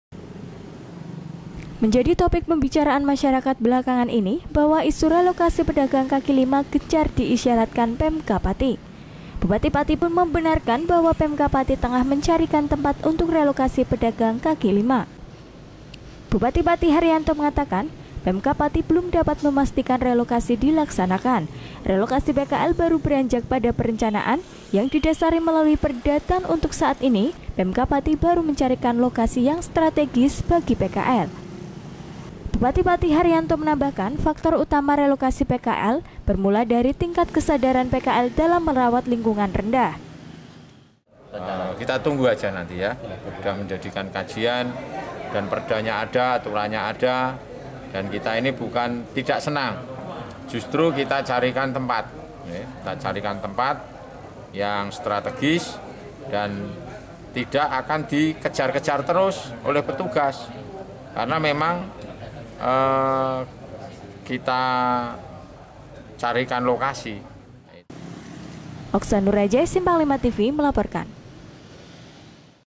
WACANA RELOKASI PKL, PEMKAB FOKUS PENCARIAN LOKASI STRATEGIS
JUDUL  : WACANA RELOKASI PKL
SUB JUDUL 1 : PEMKAB PATI FOKUS PENCARIAN LOKASI STRATEGIS
SUB JUDUL 2 : TINGKAT KESADARAN PKL UNTUK MERAWAT LINGKUNGAN MASIH RENDAH
LOKASI  : PATI – JAWA TENGAH
REPORTER  : OXA NURAJAY
TANGGAL  : 18 JULI 2017

LEAD :
PEMERINTAH KABUPATEN PATI MEMPERSIAPKAN UPAYA RELOKASI PEDAGANG KAKI LIMA DI KABUPATEN PATI/ BUPATI PATI MULAI BERGERAK MENCARIKAN LOKASI STRATEGIS YANG NANTINYA AKAN DITEMPATI PEDAGANG KAKI LIMA//
---PKG---
MENJADI TOPIK PEMBICARAAN MASYARAKAT BELAKANGAN INI/ BAHWA ISU RELOKASI PEDAGANG KAKI LIMA GENCAR DIISYARAKATKAN PEMKAB PATI// BUPATI PATI PUN MEMBENARKAN BAHWA PEMKAB PATI TENGAH MENCARIKAN TEMPAT UNTUK RELOKASI PEDAGANG KAKI LIMA//
BUPATI PATI HARYANTO MENGATAKAN/ PEMKAB PATI BELUM DAPAT MEMASTIKAN RELOKASI DILAKSANAKAN// RELOKASI PKL BARU BERANJAK PADA PERENCANAAN YANG DIDASARI MELALUI PERDA/ DAN UNTUK SAAT INI PEMKAB PATI BARU MENCARIKAN LOKASI YANG STRATEGIS BAGI PKL//
BUPATI PATI HARYANTO MENAMBAHKAN/ FAKTOR UTAMA RELOKASI PKL BERMULA DARI TINGKAT KESADARAN PKL DALAM MERAWAT LINGKUNGAN RENDAH//
WWC : H.HARYANTO, SH., MM., M.Si // BUPATI PATI //
OXA NURAJAY/ SIMPANG 5 TV/ MELAPORKAN//